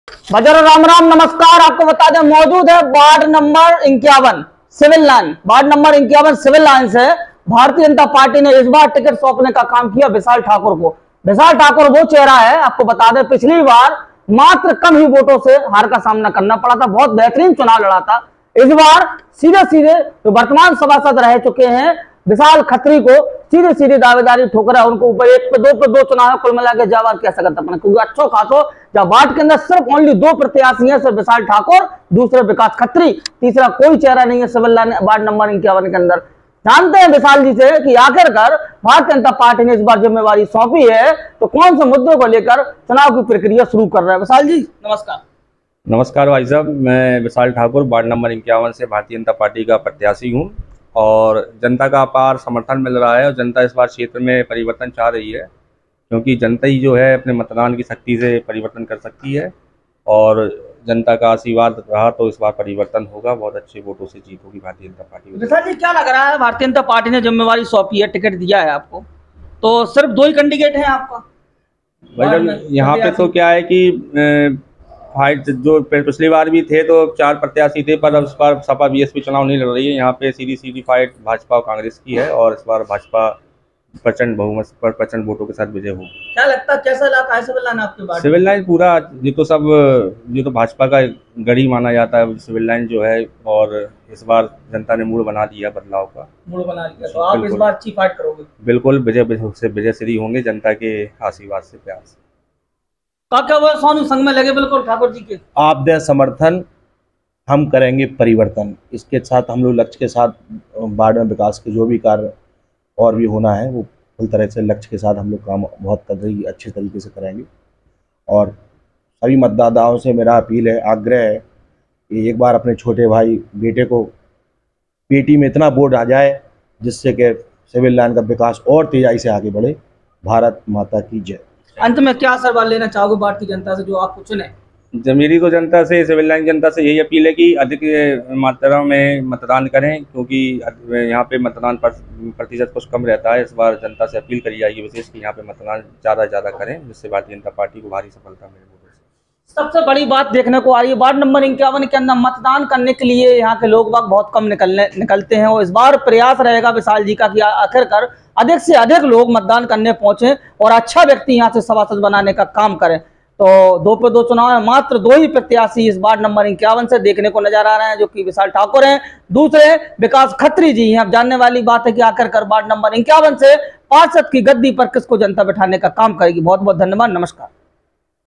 दो पे दो चुनाव वार्ड नं 51 सिविल लाईन भाजपा ने पार्षद का टिकिट देकर और कर दिया मजबूत विशाल ठाकुर को
दो पे दो चुनाव वार्ड नं 51 सिविल लाईन भाजपा ने पार्षद का टिकिट देकर और कर दिया मजबूत विशाल ठाकुर को न्यूज़ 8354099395